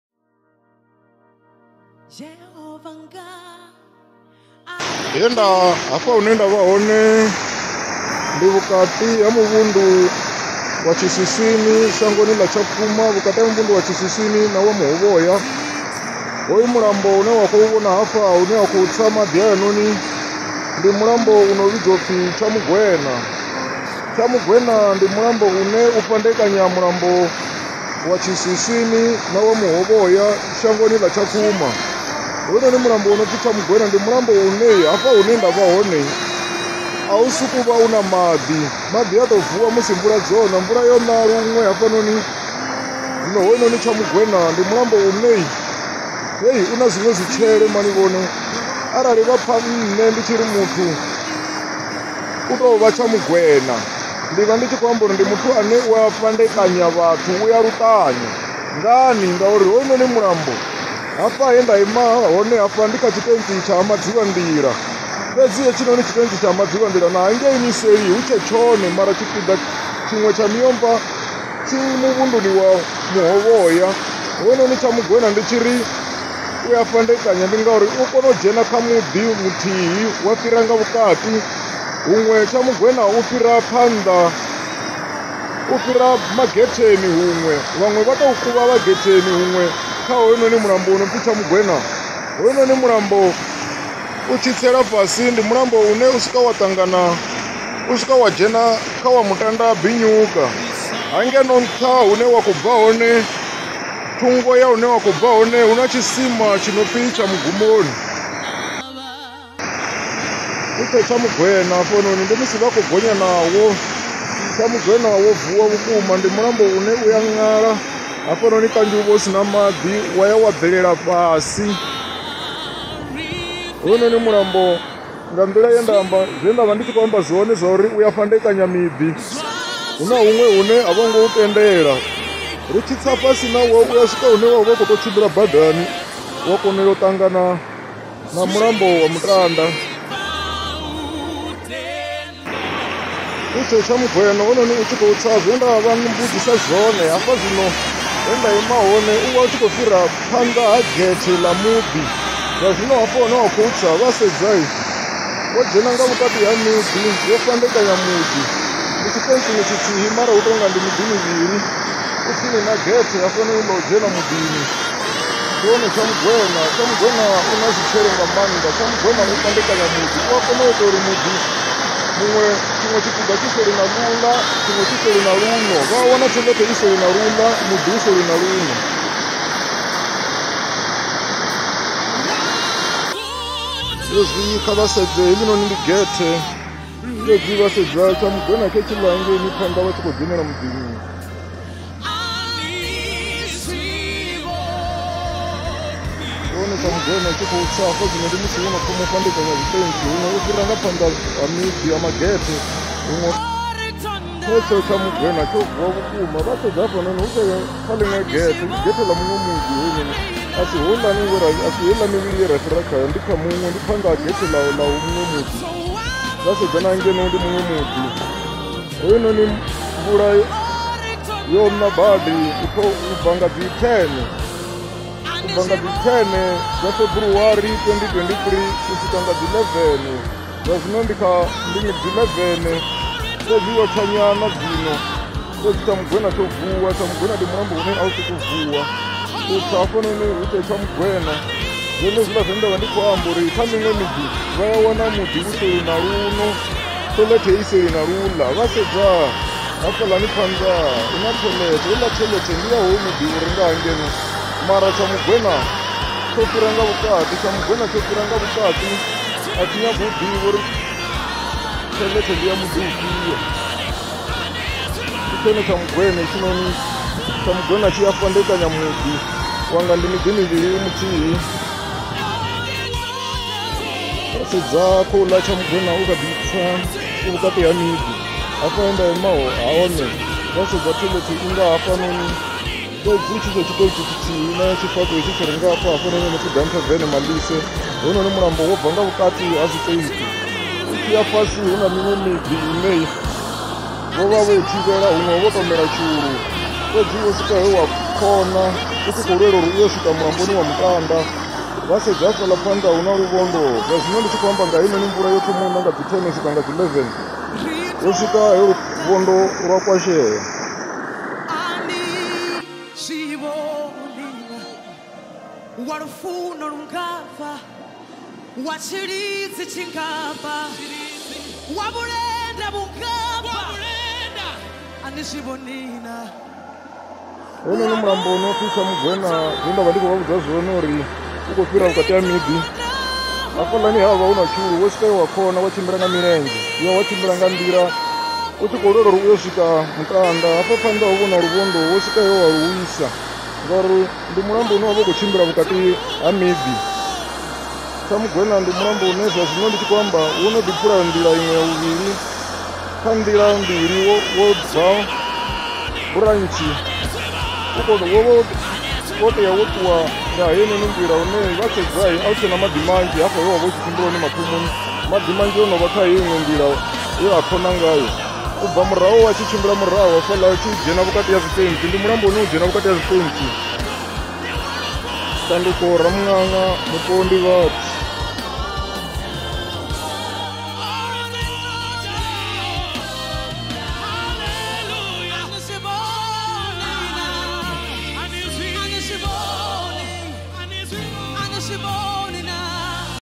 TSHAMUGWENA
TSHAMUGWENA ndi mulambo u ne wa fhandekanya zwitentzi.